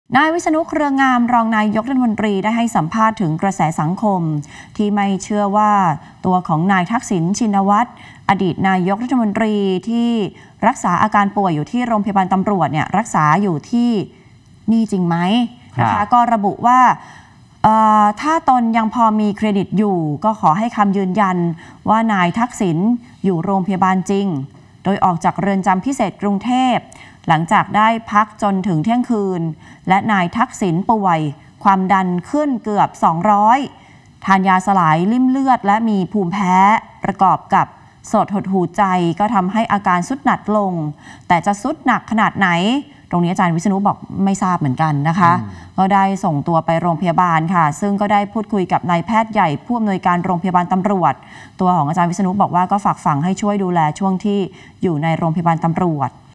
“ทักษิณ”ขออภัยโทษ “วิษณุ”เผยเอกสารถึงรัฐบาลแล้ว
"วิษณุ เครืองาม” เผย "ทักษิณ ชินวัตร" ได้ขอพระราชทานอภัยโทษแล้ว แต่ไม่สามารถบอกได้ว่า จะใช้เวลานานเท่าไหร่ ทุกอย่างเป็นไปตามกระบวนการ